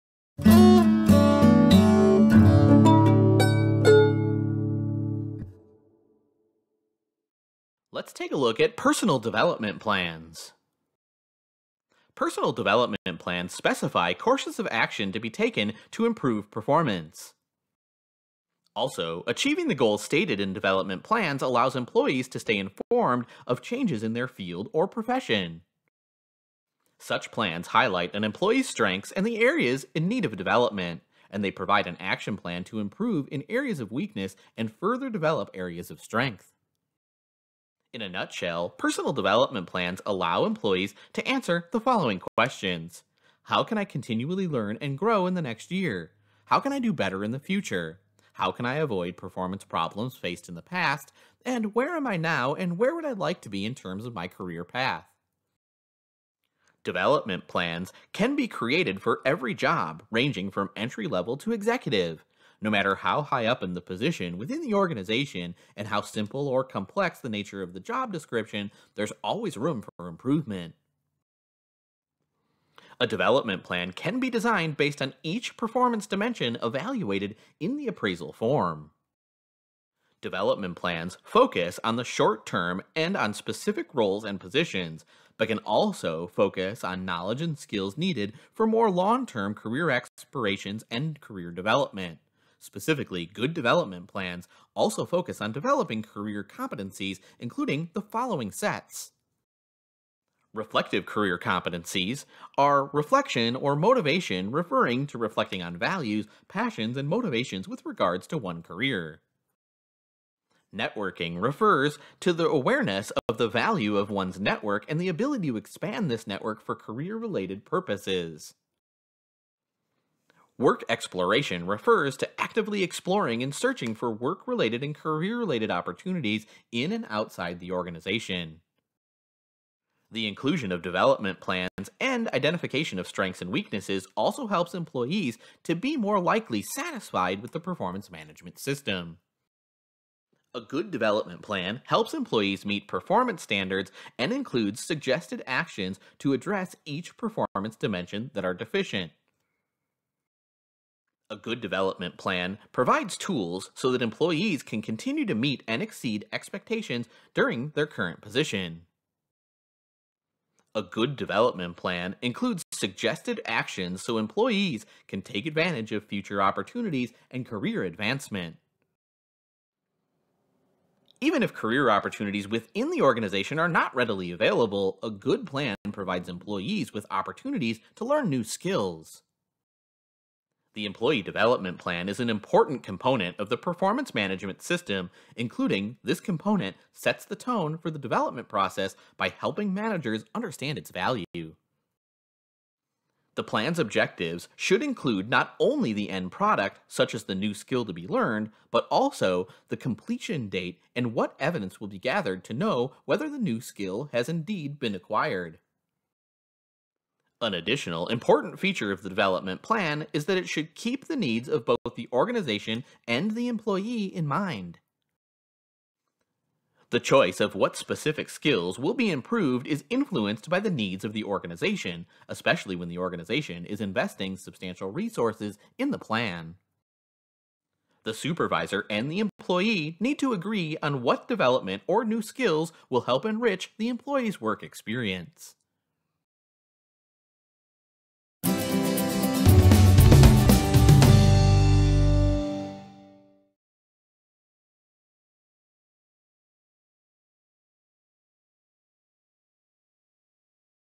Personal Development Plans
Personal development plans specify courses of action to be taken to improve performance. Also, achieving the goals stated in the development plan allows employees to stay informed of changes in their field or profession. Such plans highlight an employee’s strengths and the areas in need of development, and they provide an action plan to improve in areas of weaknesses and further develop areas of strength.

Development plans can be created for every job, ranging from entry level to the executive suite (e.g., CEO, CFO). No matter how high up the position within the organization and how simple or complex the nature of the job in question, there is always room for improvement. A development plan can be designed based on each of the performance dimensions evaluated on the appraisal form. For example, if the performance dimension “communication” is rated as substandard, this area would be included in the development plan.

Development plans focus on the short term and on specific roles and positions, but also on the knowledge and skills needed for more long-term career aspirations and career development. The inclusion of development plans and identification of strengths and weaknesses also helps employees to be more likely to be satisfied with the performance management system.